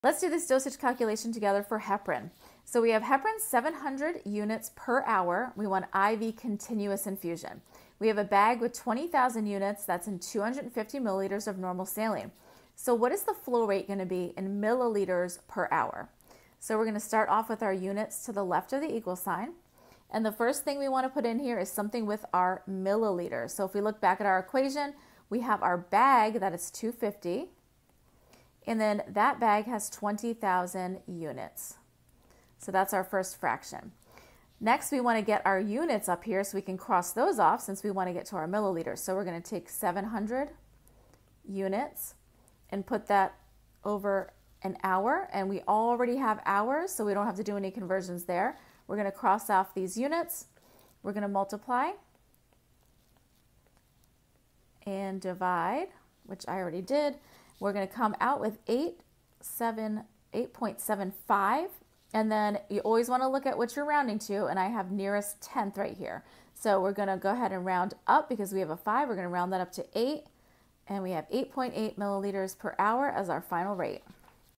Nursing Dosage Calculations Practice
Let's do a dosage calculation practice question together.

Heparin 700 units/hr IV continuous infusion. The available bag contains 20,000 units of Heparin in 250 mL of NS. What will the flow rate be in mL/hr? Round to the nearest tenth.